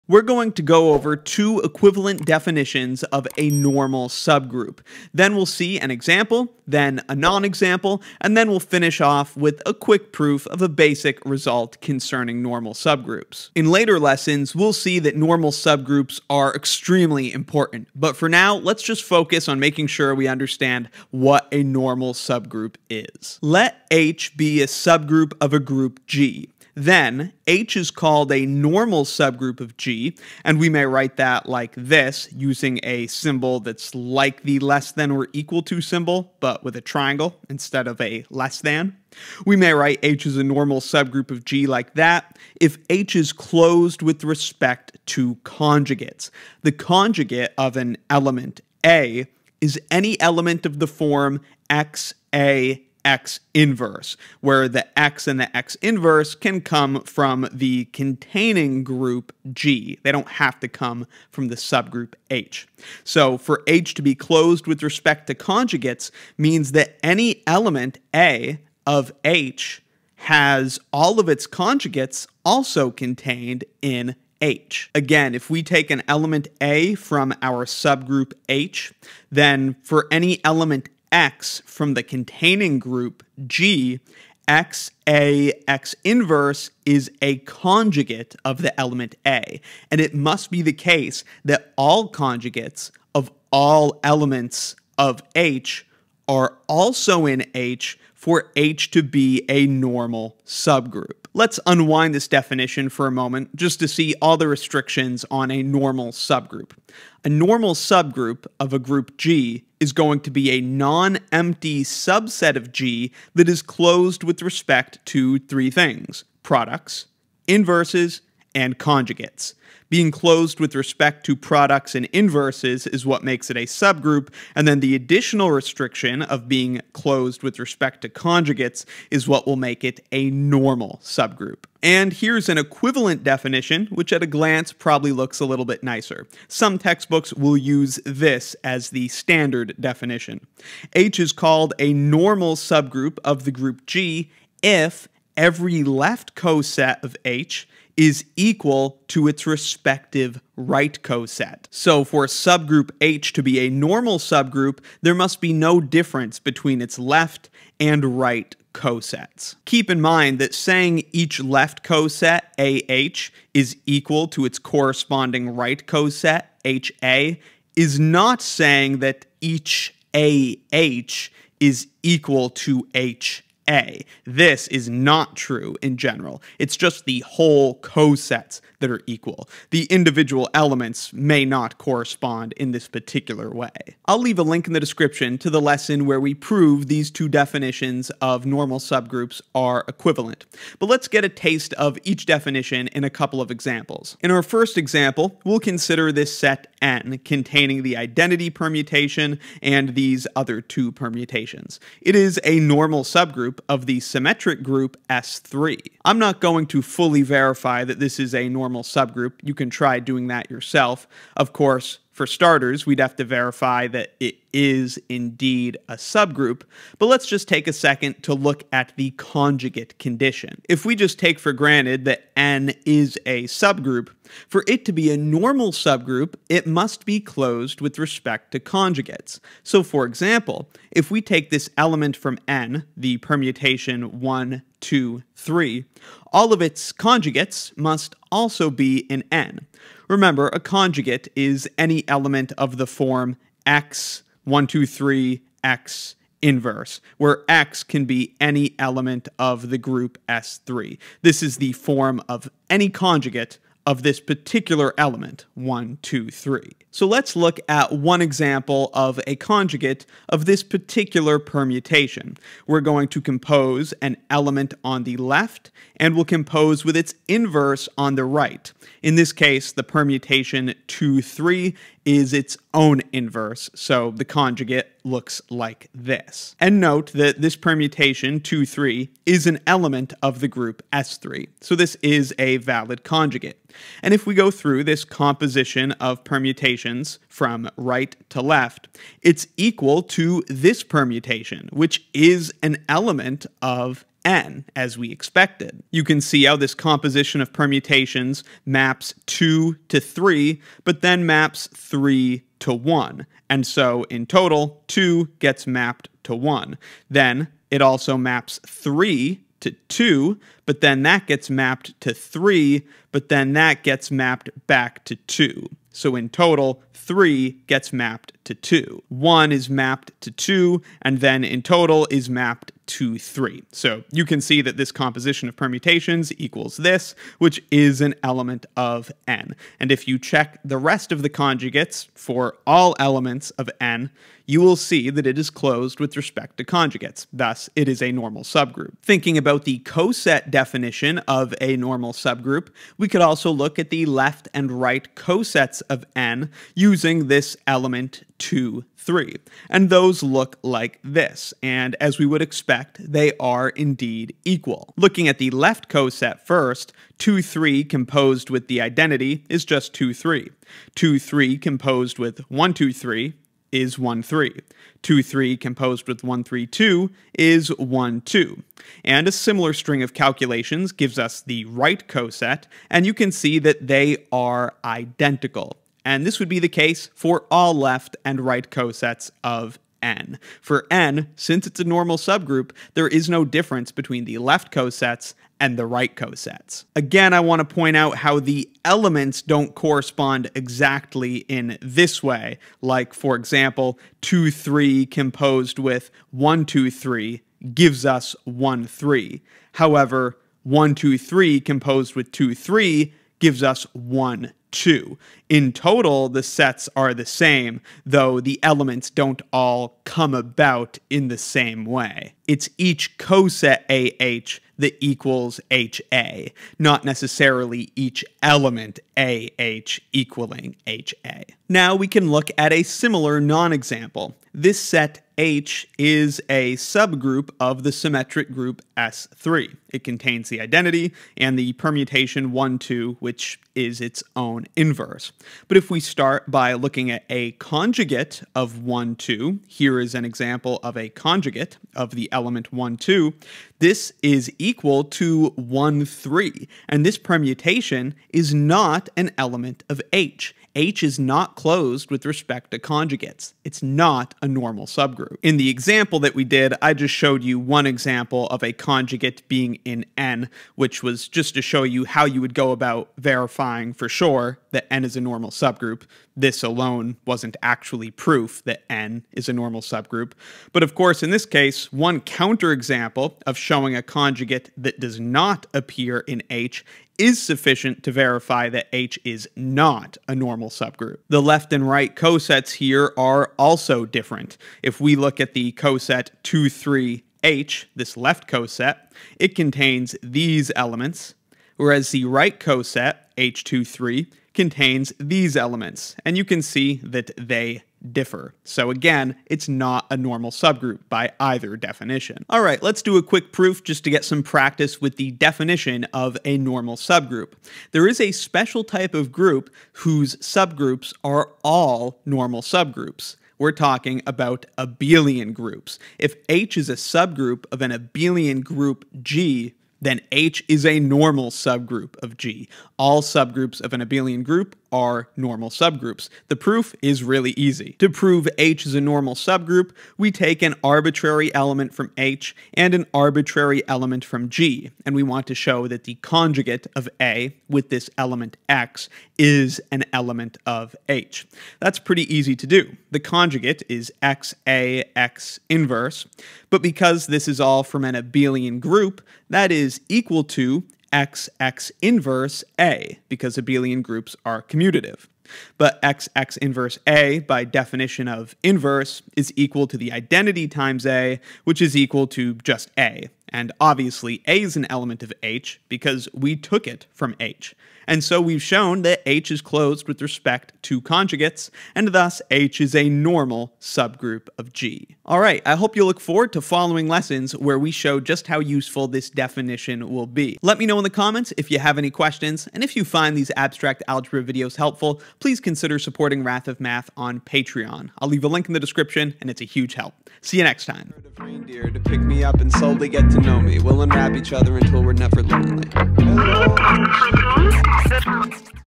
Definition of Normal Subgroups | Abstract Algebra
We introduce normal subgroups with their conjugate definition and the coset definition. We'll see examples of normal subgroups and nonexamples of normal subgroups, and we finish with a proof that every subgroup of an abelian group is a normal subgroup. A subgroup H of a group G is normal if H is closed with respect to conjugates, or equivalently if each left coset aH is equal to each right coset Ha for every a in G.